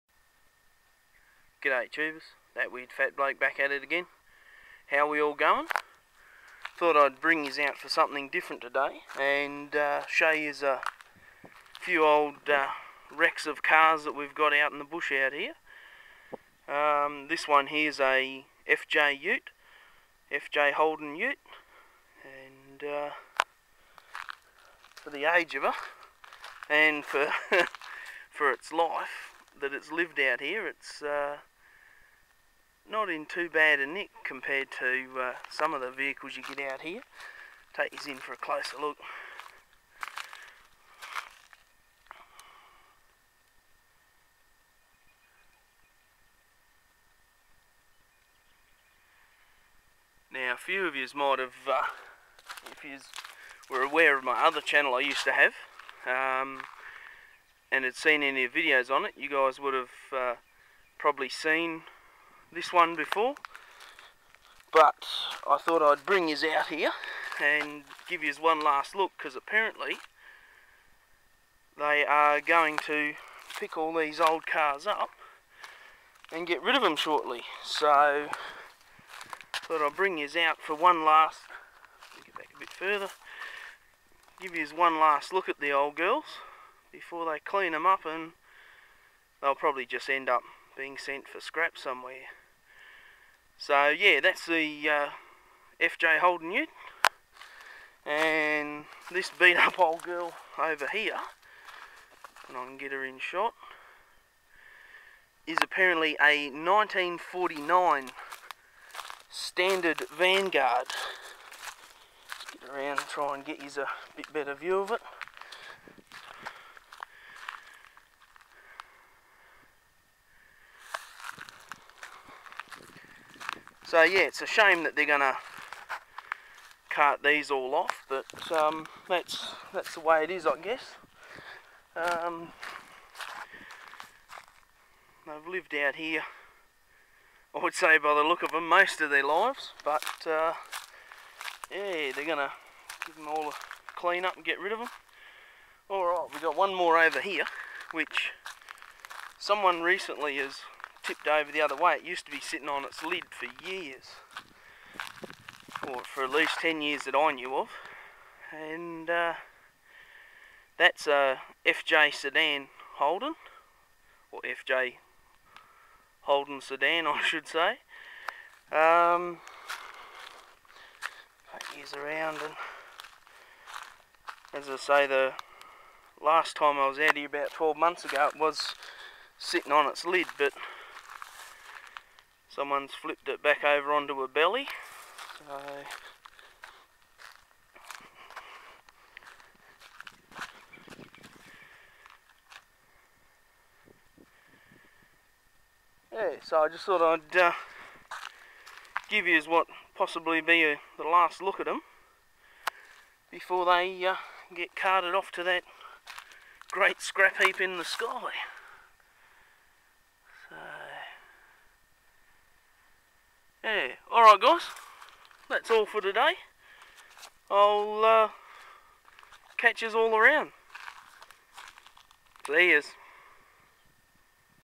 BLOKESIZE™ RELICS #1... FJ Holden Ute, FJ Holden Sedan, 1949 Standard Vanguard
Some old cars that have been abandoned out in the bush near to where I live.